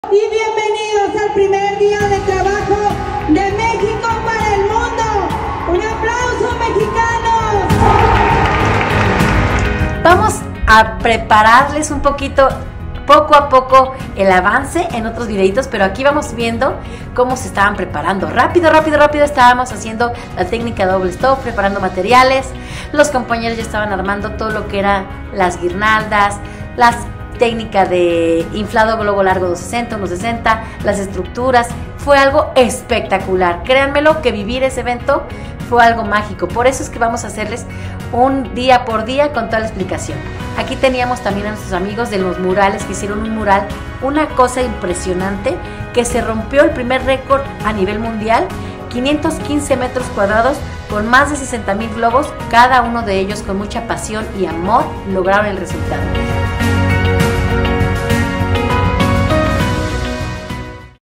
¡Ganamos record Guinness! 🎈🎉
¡Acompañanos a ver un poco de cómo se vivió el día 1 en este gran evento donde obtuvimos récord Guinness!
Gracias a nuestro increíble equipo y a todos los que formaron parte de esta colorida hazaña.